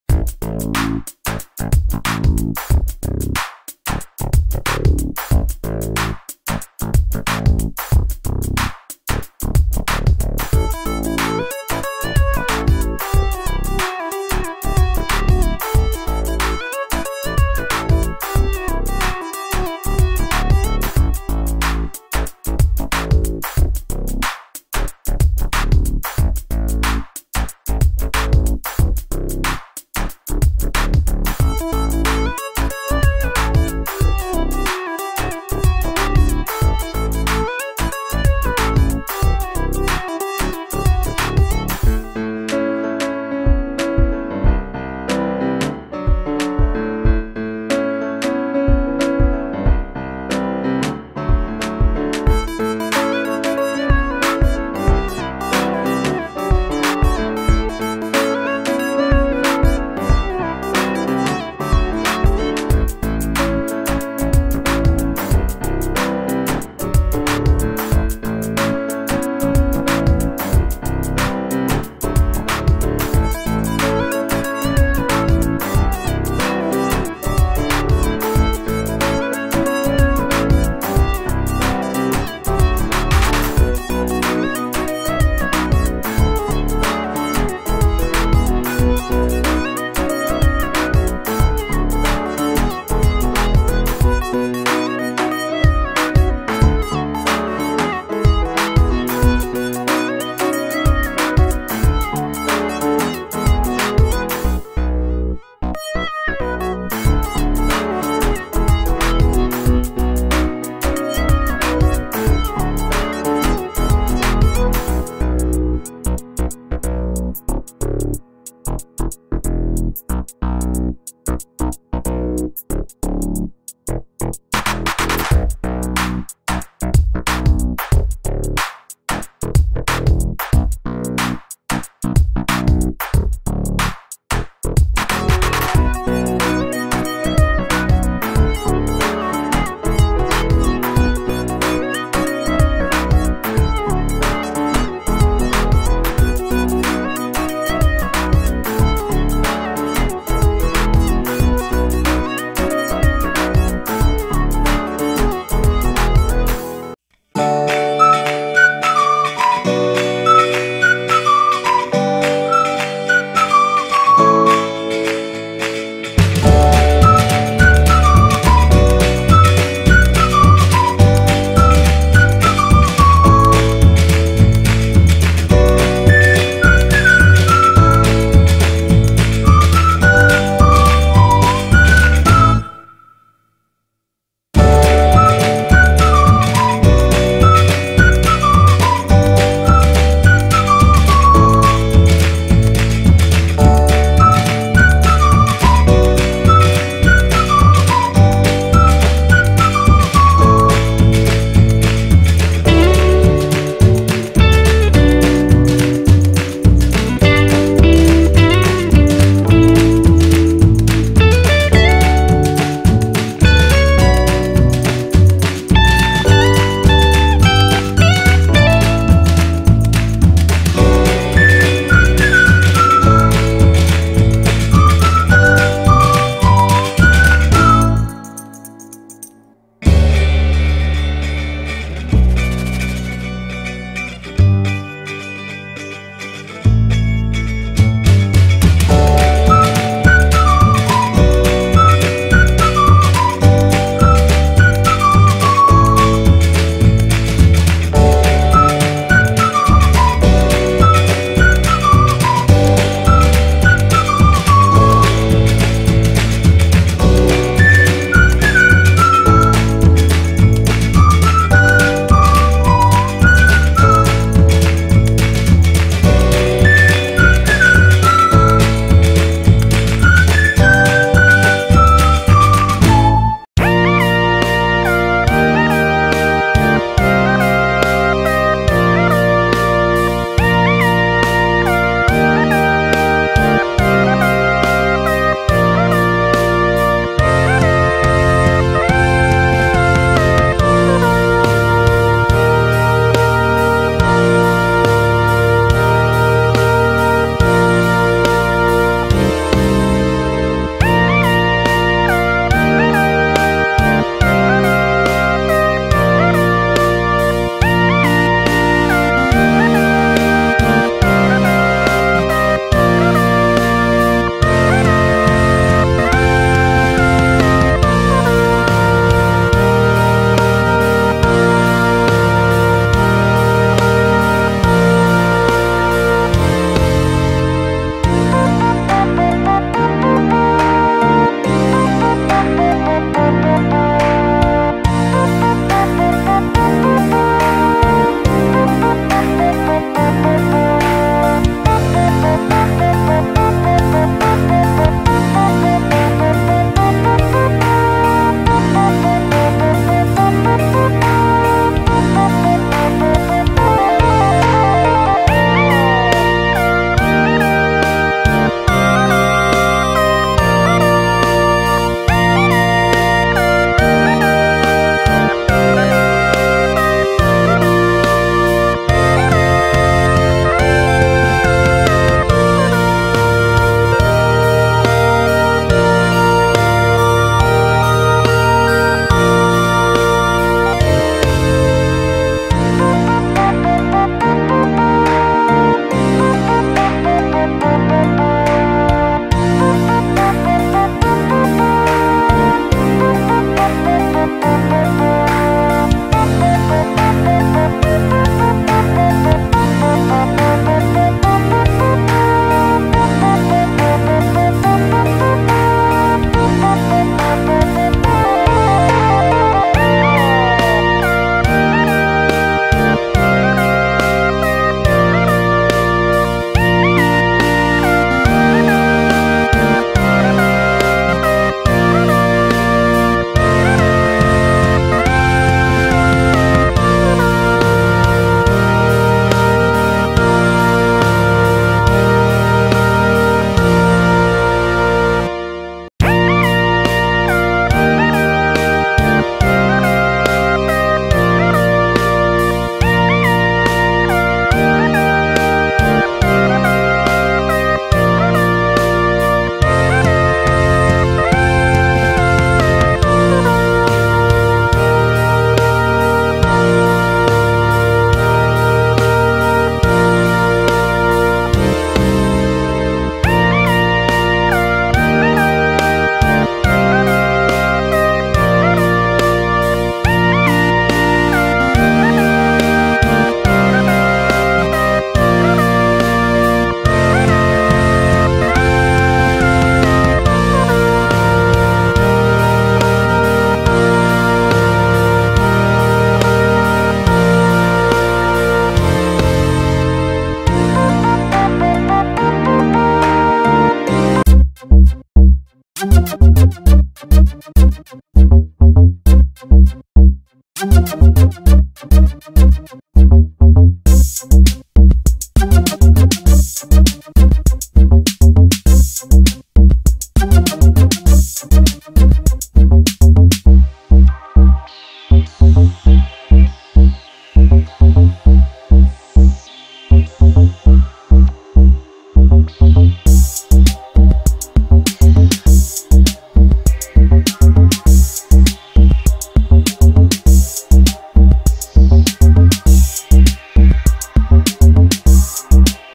Cetatea Sorocii din chibrite "fortress made of matches"
In this video I decided to make an experiment and did it - I built a fortress of matches. Matches are very good material and decided to check what will happen if the construction of the fortress will be made. This experiment was successful, it turned out very beautiful. This is a small lifhak for those who want to make a beautiful decoration.